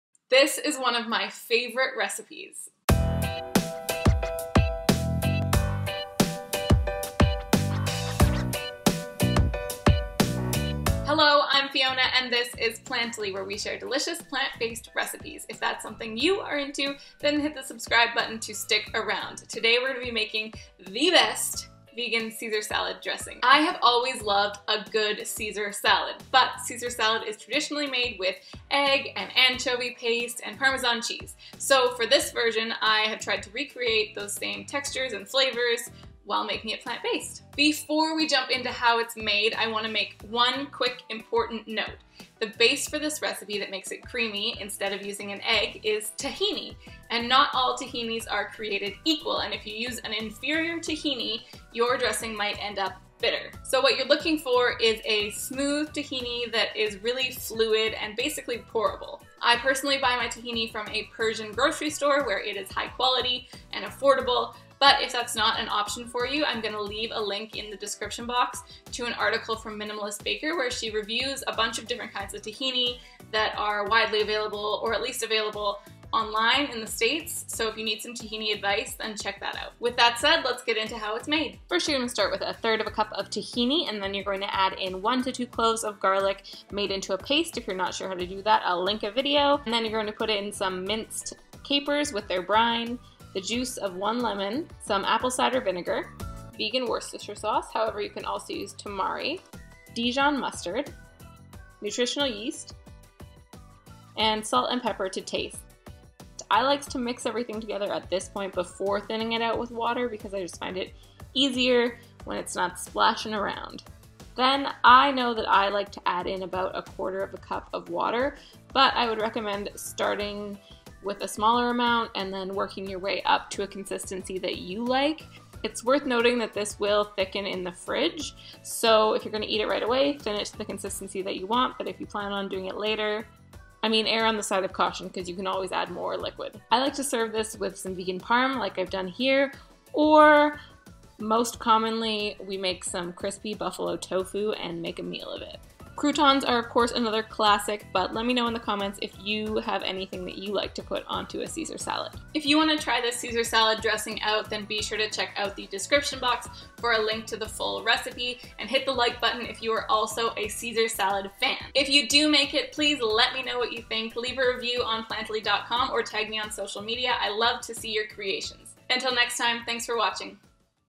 Vegan Caesar Dressing Recipe | nut-free + dairy-free salad dressing
This vegan caesar dressing recipe is creamy, tangy, and super quick to make. It’s nut-free, oil-free, dairy-free, egg-free, and you don’t even need to use a blender.